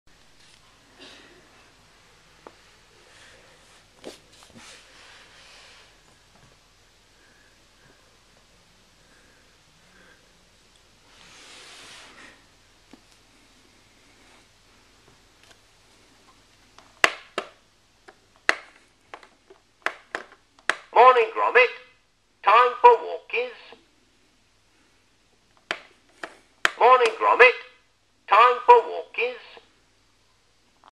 Wallace and Gromit Alarm Clock Preview
Another one of my rare Wallace and Gromit alarm clocks, more to come!